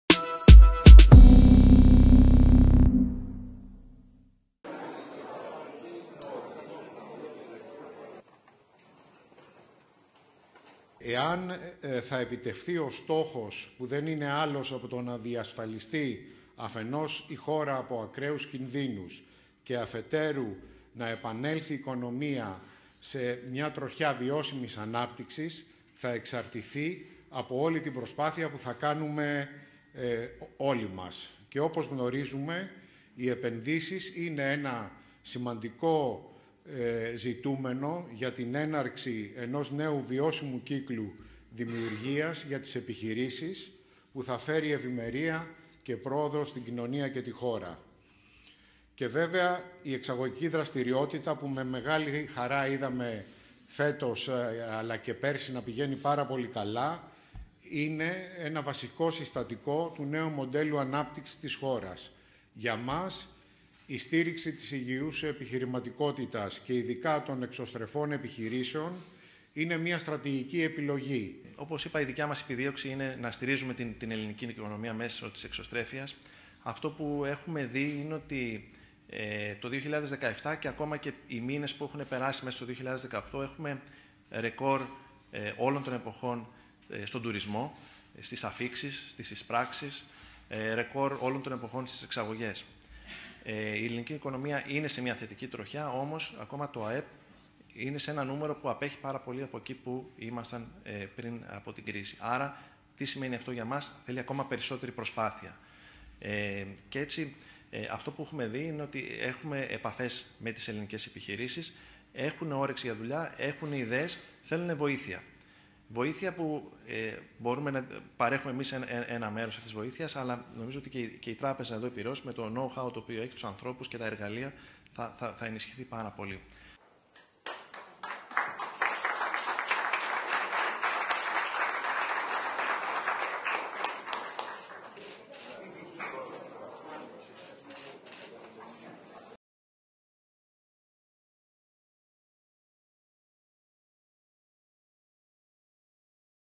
Υπογραφή συμφώνου συνεργασίας Τράπεζας Πειραιώς και Enterprise Greece
Τελετή υπογραφής Συμφώνου Συνεργασίας, πραγματοποιήθηκε στα Κεντρικά Γραφεία του Ομίλου Πειραιώς. Πηγή ΑΠΕ-ΜΠΕ / Κ. Σοροβός

Δηλώσεις
1) Χρήστος Μεγάλου, Διευθύνων Σύμβουλος Τράπεζας Πειραιώς
2) Ηλίας Αθανασίου, Διευθύνων Σύμβουλος Enterprise Greece